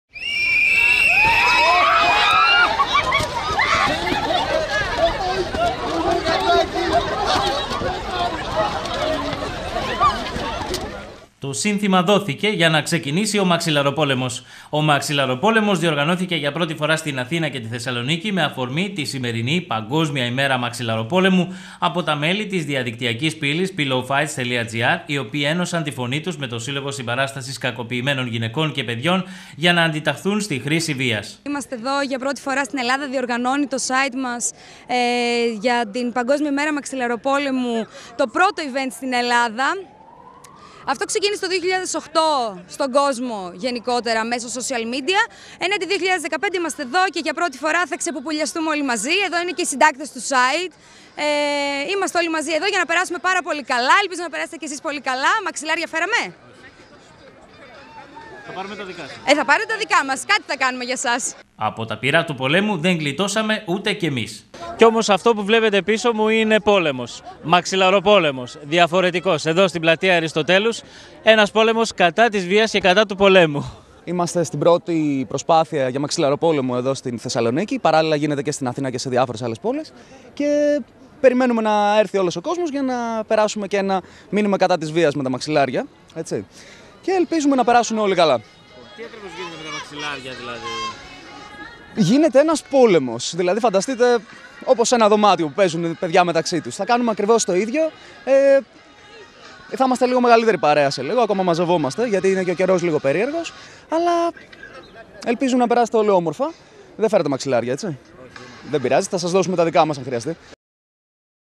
ΜΑΞΙΛΑΡΟΠΟΛΕΜΟΣ ΣΤΗΝ ΠΛΑΤΕΙΑ ΑΡΙΣΤΟΤΕΛΟΥΣ 5/4/2015
Μαξιλάρια σε πολλά σχήματα και χρώματα «επιστράτευσαν» νεαροί κάτοικοι της Αθήνας και της Θεσσαλονίκης οι οποίοι θέλησαν να στείλουν μήνυμα ενάντια στη βία. Επιδίωξη των διοργανωτών είναι να καθιερωθεί η σημερινή ημέρα και στη χώρα μας, όπως ισχύει εδώ και αρκετά χρόνια και σε πόλεις του εξωτερικού.